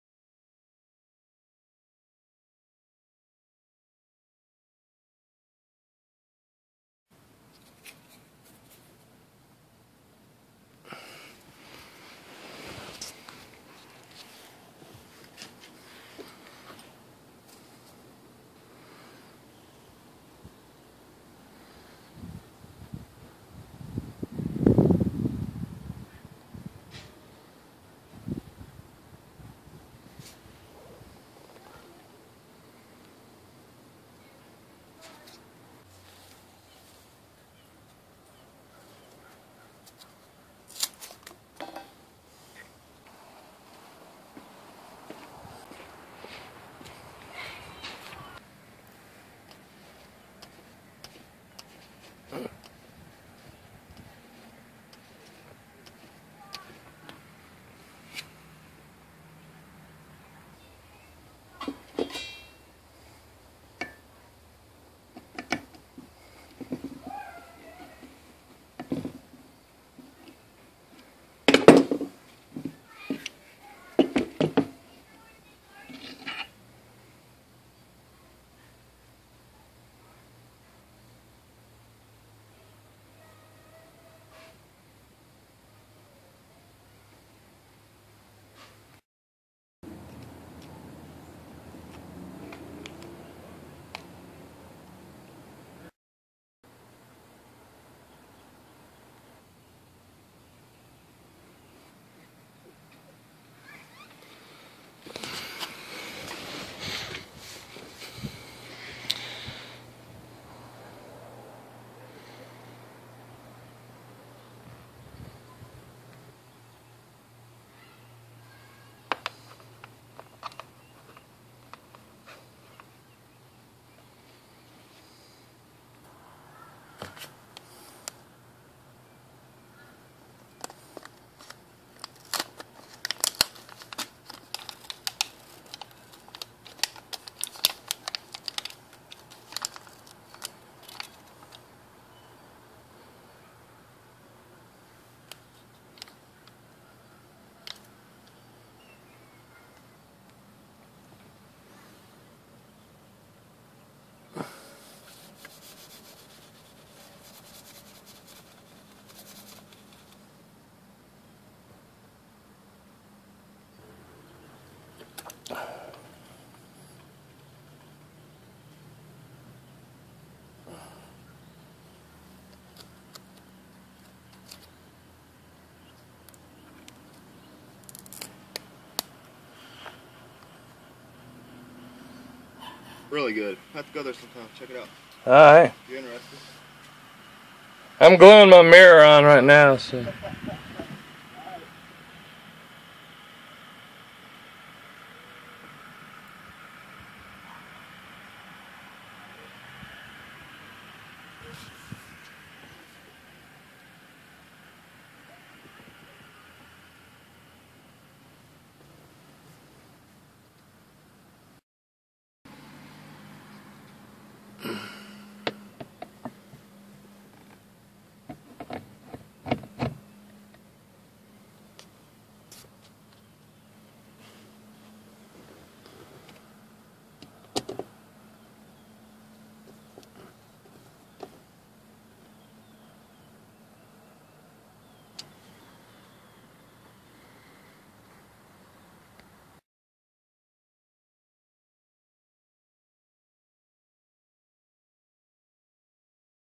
How to glue a rear-view mirror on using a magnet
Over the years I have installed a lot of rear-view mirrors that have fallen off.
Sometimes during the act of holding the mount plate it can slip causing it to be crooked.
I had the idea to use a hard disk drive neodymium magnet to aide in the process.